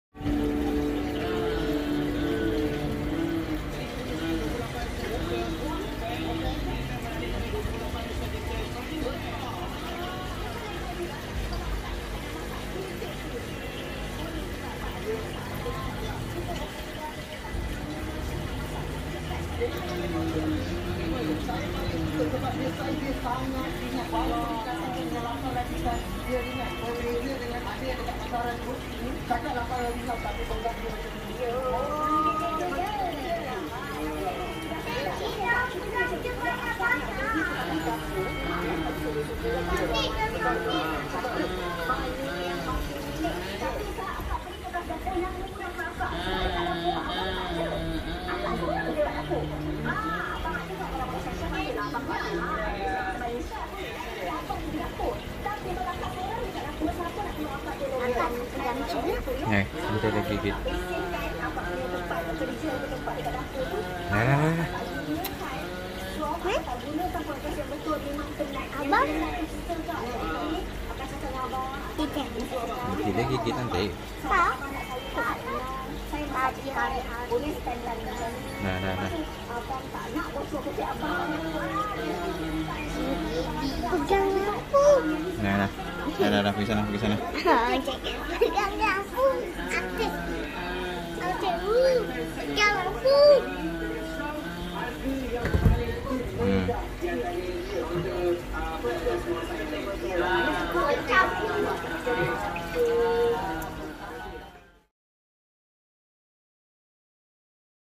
Si Buta - Arowana Malaysia Golden
kisah disebalik si buta, Pemilik asalnya tidak dapat beberapa bulan kerana tidak dapat merentas negeri (PKP) sehingga menyebabkannya buta kerana kekotoran air. Dan akhirnya pemillik asalnya memberikan tangungjawab sepnuhnya kepadaku untuk menjaganya. Si buta tiada bayaran (pecuma). Tetapi kasih sayang menjagamu tetap sama seperti jiran di atasmu Si Sado. Kini hampir 6 bulan dalam jagaan. Syukur, dia semakin sihat sehingga kini